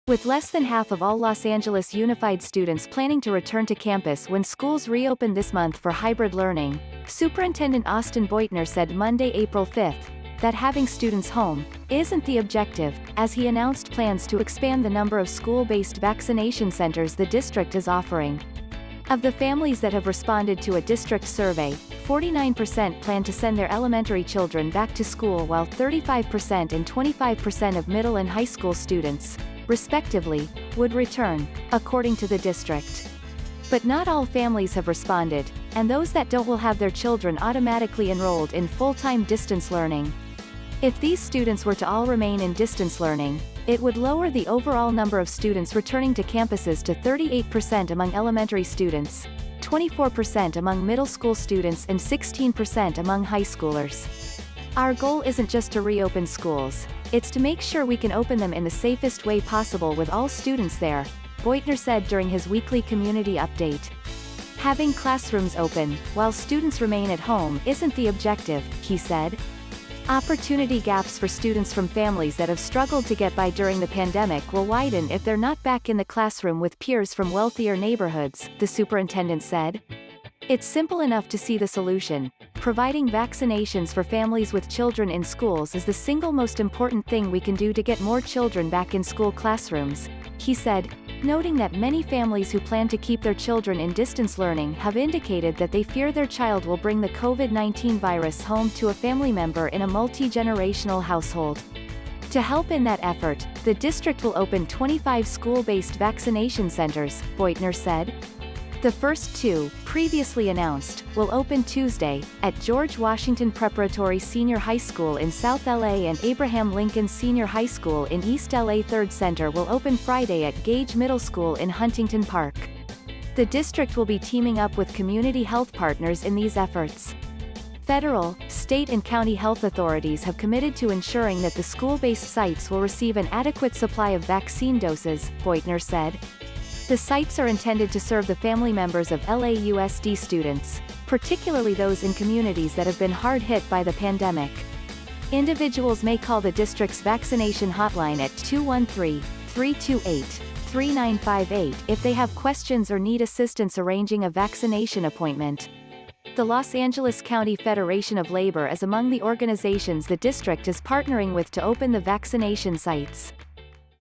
LAUSD to open 25 school based vaccination sites for district families
With less than half of all Los Angeles Unified students planning to return to campus when schools reopen this month for hybrid learning, Superintendent Austin Beutner said Monday, April 5, that having students home “isn’t the objective” as he announced plans to expand the number of school-based vaccination centers the district is offering. Of the families that have responded to a district survey, 49% plan to send their elementary children back to school while 35% and 25% of middle and high school students, respectively, would return, according to the district. But not all families have responded, and those that don’t will have their children automatically enrolled in full-time distance learning. If these students were to all remain in distance learning, it would lower the overall number of students returning to campuses to 38% among elementary students, 24% among middle school students and 16% among high schoolers.“Our goal isn’t just to reopen schools; it’s to make sure we can open them in the safest way possible with all students there, ” Beutner said during his weekly community update. Having classrooms open “while students remain at home isn’t the objective, ” he said. Opportunity gaps for students from families that have struggled to get by during the pandemic will widen if they’re not back in the classroom with peers from wealthier neighborhoods, the superintendent said.“It’s simple enough to see the solution: providing vaccinations for families with children in schools is the single most important thing we can do to get more children back in school classrooms, ” he said, noting that many families who plan to keep their children in distance learning have indicated that they fear their child will bring the COVID-19 virus home to a family member in a multigenerational household. To help in that effort, the district will open 25 school-based vaccination centers, Beutner said. The first two, previously announced, will open Tuesday — at George Washington Preparatory Senior High School in South L. A. and Abraham Lincoln Senior High School in East L. A third center will open Friday at Gage Middle School in Huntington Park. The district will be teaming up with community health partners in these efforts. Federal, state and county health authorities have committed to ensuring that the school-based sites will receive an adequate supply of vaccine doses, Beutner said. The sites are intended to serve the family members of LAUSD students, particularly those in communities that have been hard hit by the pandemic. Individuals may call the district’s vaccination hotline at if they have questions or need assistance arranging a vaccination appointment. The Los Angeles County Federation of Labor is among the organizations the district is partnering with to open the vaccination sites.